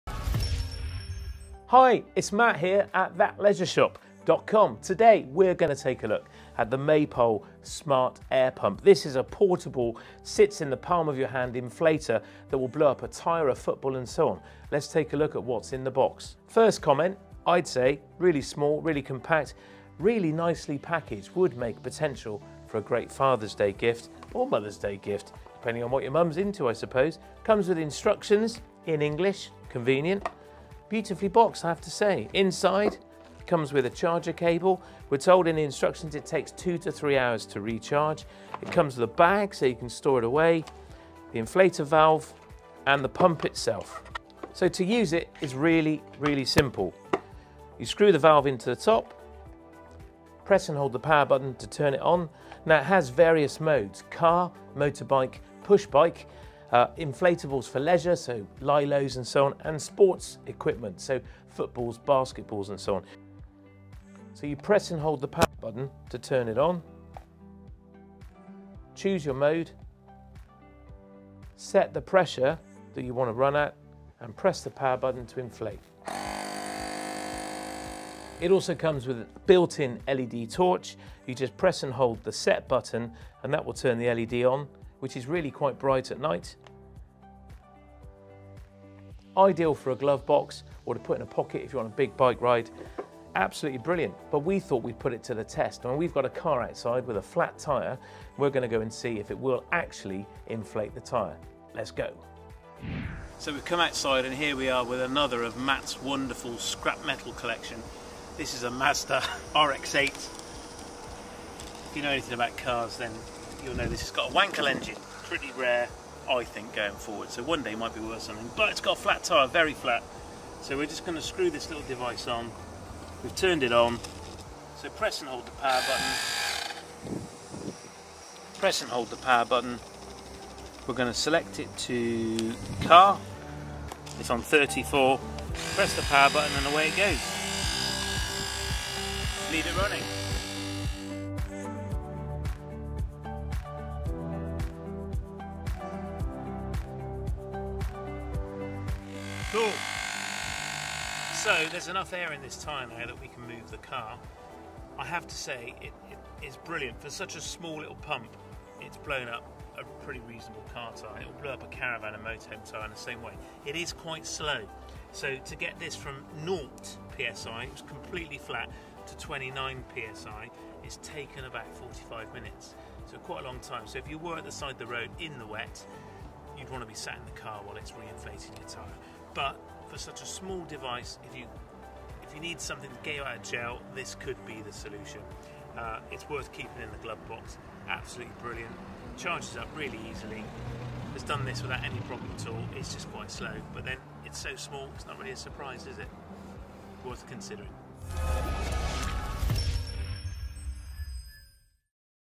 Maypole Motorhome and Campervan Smart Air Pump
For high force and quick inflation, look no further than Maypole Smart Air Pump – an all-rounder suitable for bikes, cars, motorcycles, inflatables and sports balls.

This nifty pump will make setting up camp a whole lot easier and its compact and lightweight design will allow you to carry it with you wherever you go.

The handheld rechargeable air pump has a powerful 2000mAh lithium-ion battery and can even be used as a power bank for mobile phone charging.

With a large LCD display, you’ll be able to check tyre pressure at any time – an essential tool for safety and fuel efficiency. The Maypole Smart Air pump covers it all and is incredibly convenient to use.

That Leisure Shop, enjoy the journey.